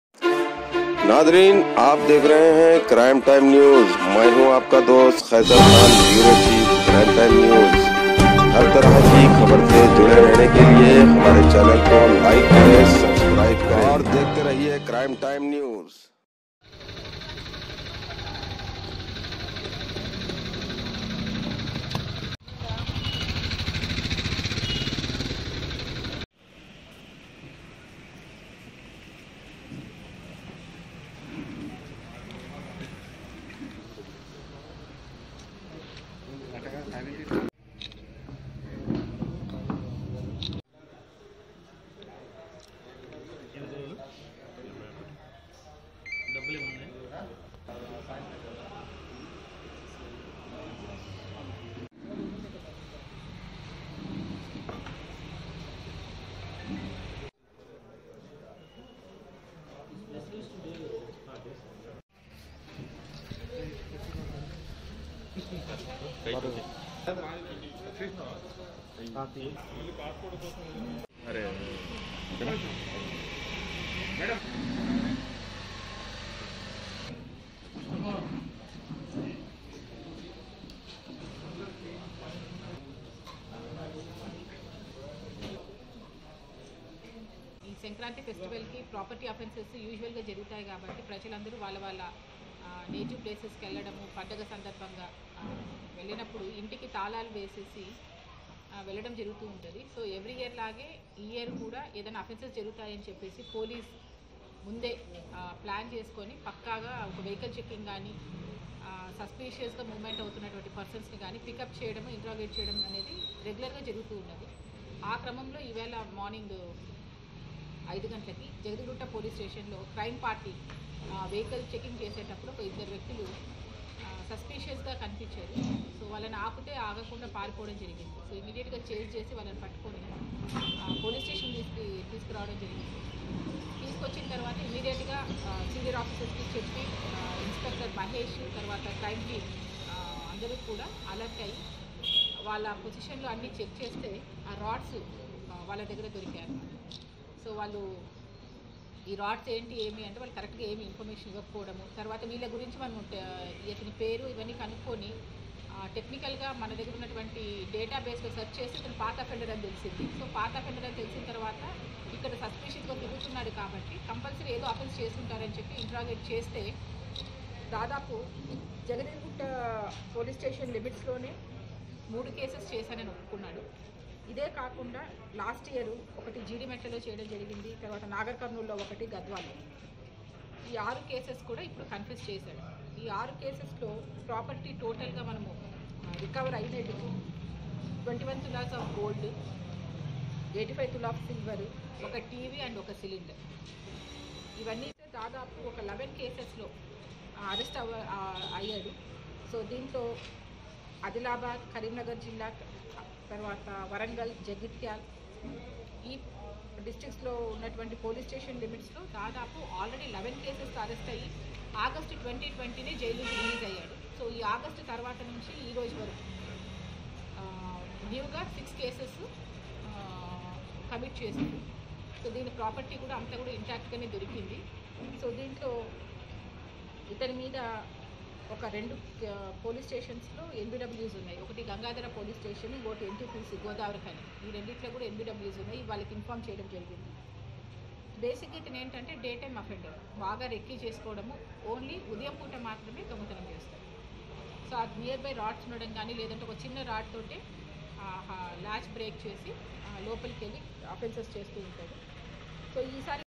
Crime Time News... Jagadgirigutta Police Arrested 2 Burglary Offender Who Committed Several Thefts
Jagadgirigutta Police Arrested 2 Burglary Offender Who Committed Several Thefts And Seized 21 Pounds Gold Jewellery, 80 Pounds Silver Jewellery, One Motorcycle, Cylinder, Tv And Phone Were Seized From The Accused Total Worth Value Around Rs.12.25 Lakhs By DCP Balangar PV Padmaja

By Sub Editor Shoaib Khan...